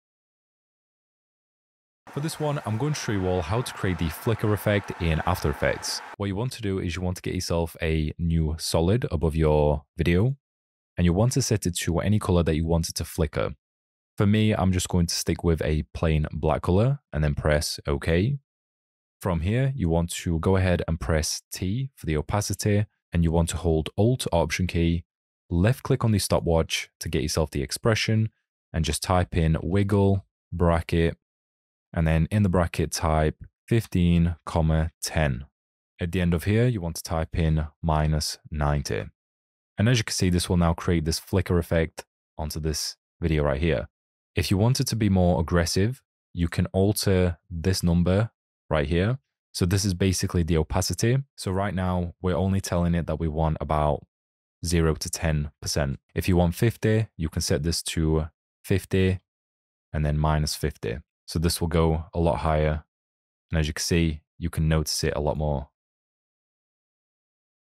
How To Easily Create a Flicker Effect in After Effects
Easily create this random flicker effect where the layer opacity is flickering the opacity levels in Adobe After Effects. Creating a flicker effect in After Effects is really simple and can be done with any layer opacity using Adobe After Effects. Layer flicker effect is done with layer opacity or any solid you want to flicker in your After Effects project.